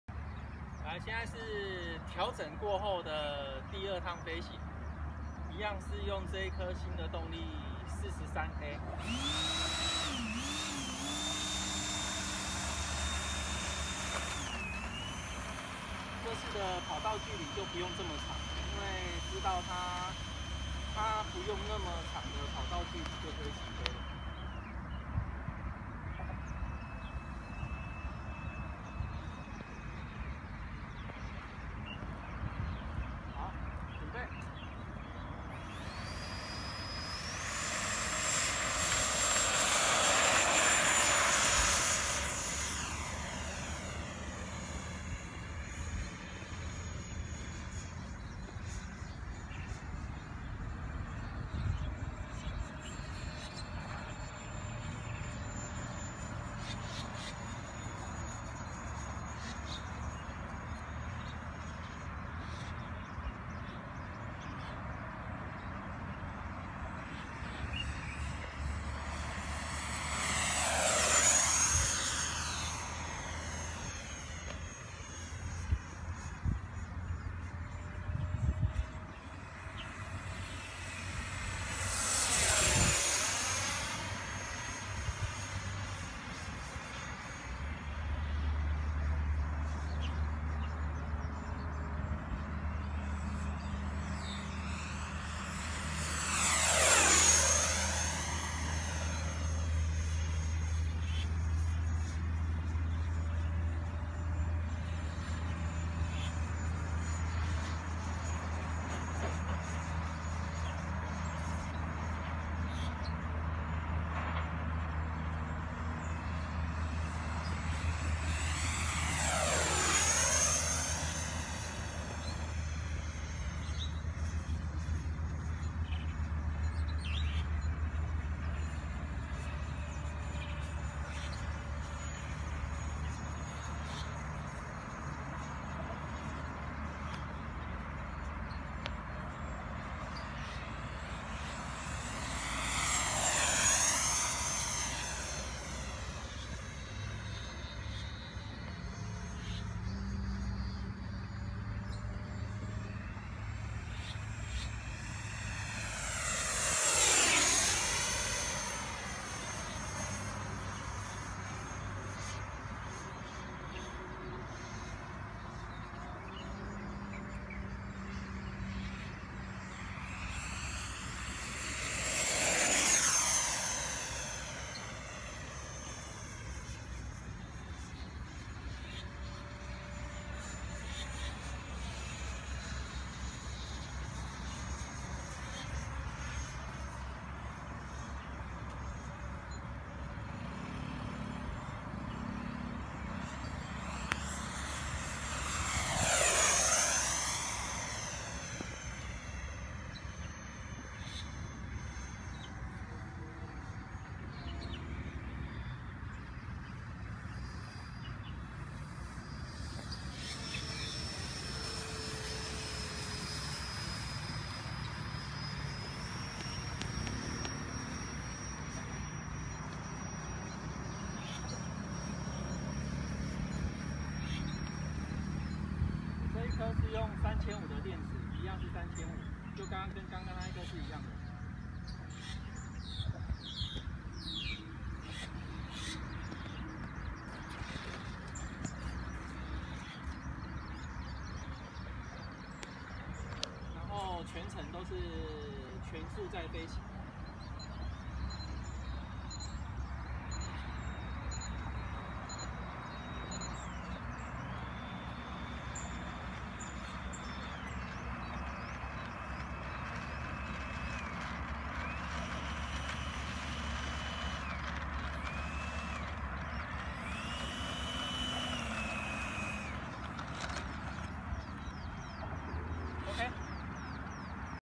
70mm 新動力測試 電流43A 推力2.0-2.1Kg 第二趟飛行
使用JH 6S 3500 35C